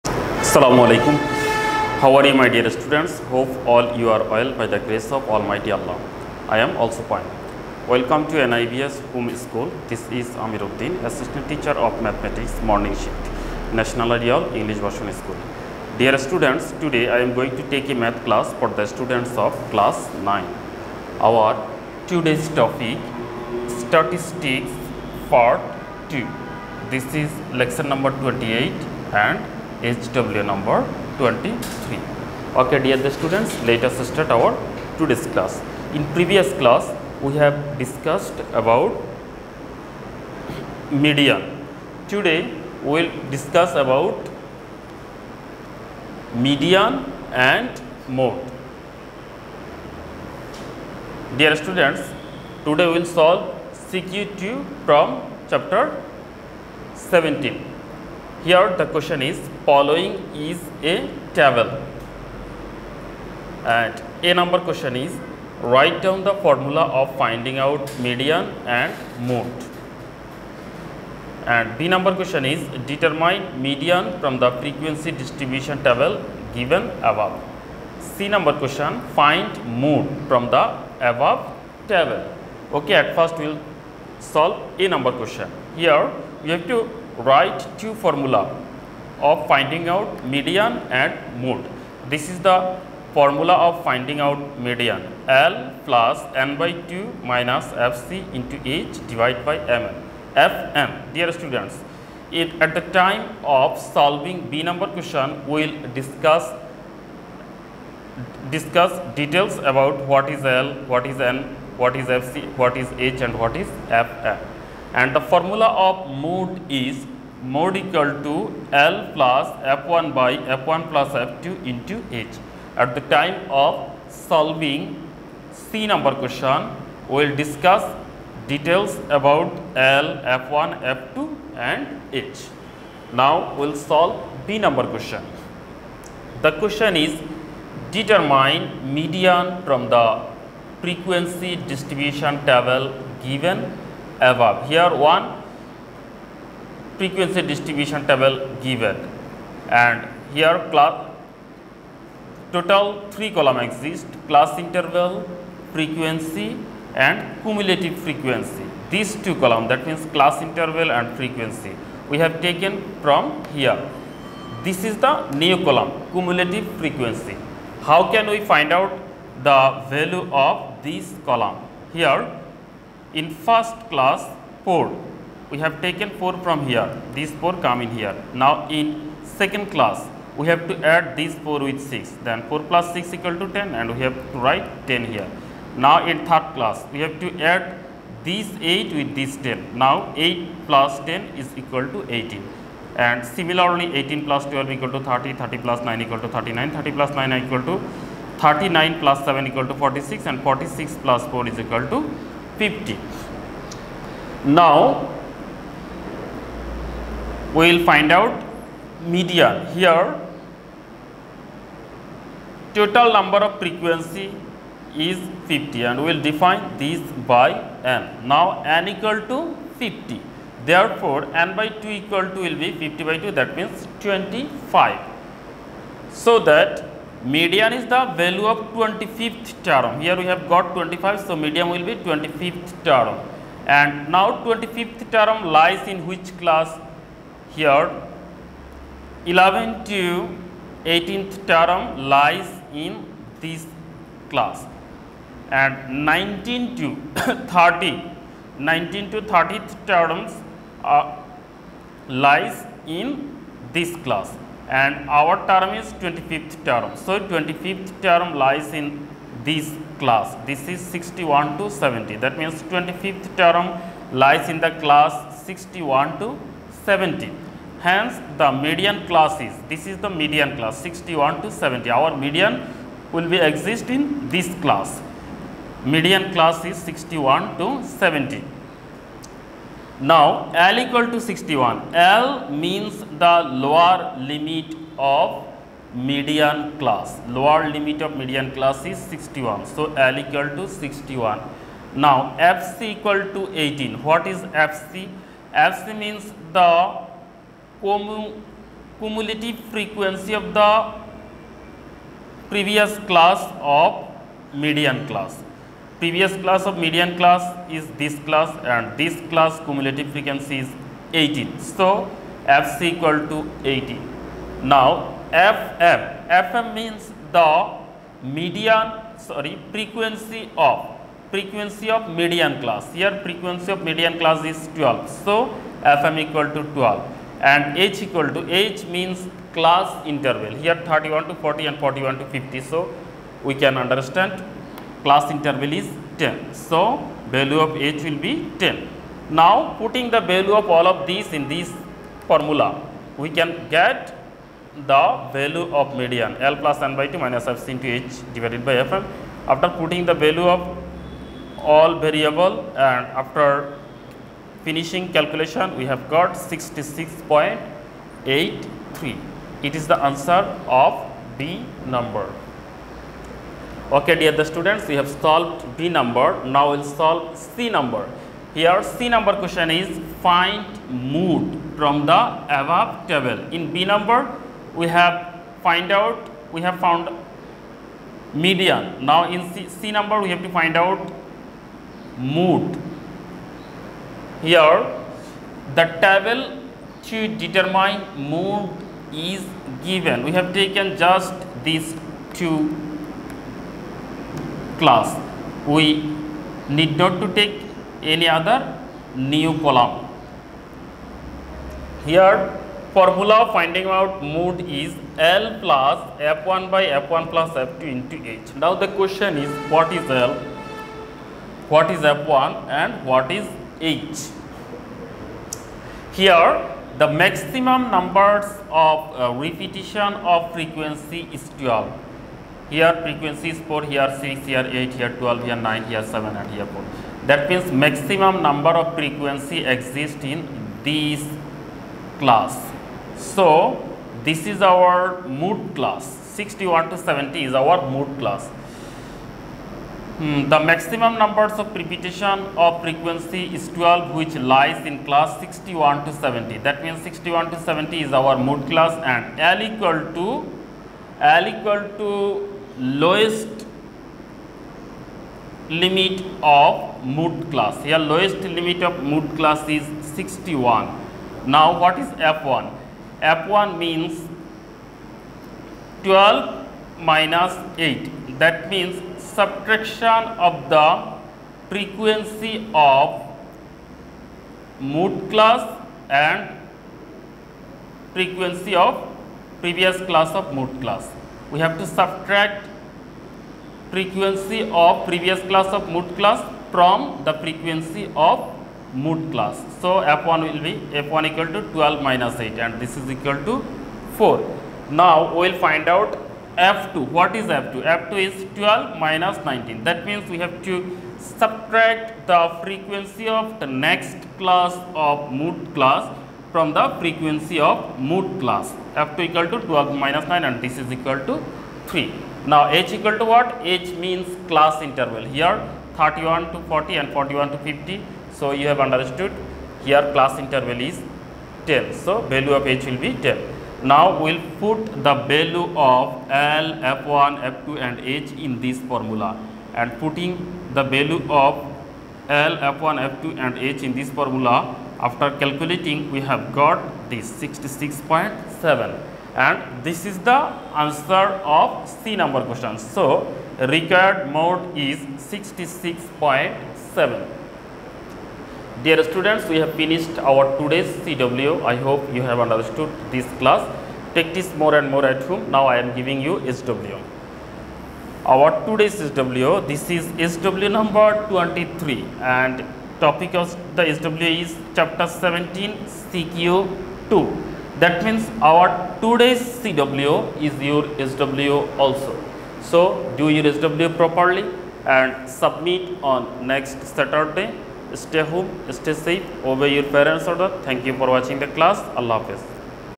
Class: Nine, Subject: Mathematics (Lecture- 28), Topic: Statistics (Part-2)
Class: Nine
Subject: Mathematics (Lecture- 28)
Topic: Statistics (Part-2)
Teacher: Amir Uddin